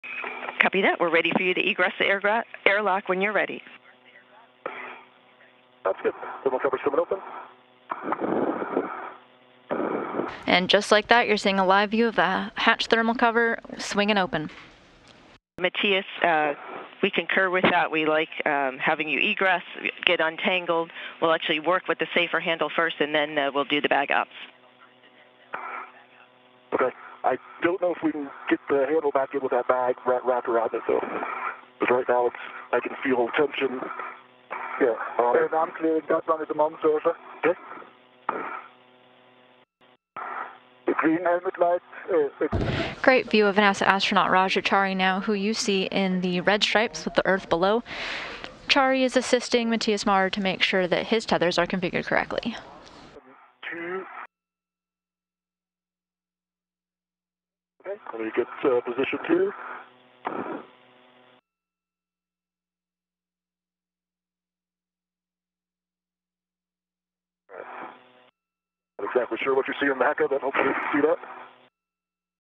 Astronauts replace radiator hose on Space Station
(23 Mar 2022) Astronauts on Wednesday worked to install hoses on a valve module, a power and a data cable and replace an external camera during a spacewalk at the International Space Station.
Expedition 66 Flight Engineers Raja Chari of NASA and Matthias Maurer of ESA (European Space Agency) began the spacewalk at 8:32 a.m. EDT to install hoses on a radiator valve to support temperature regulation.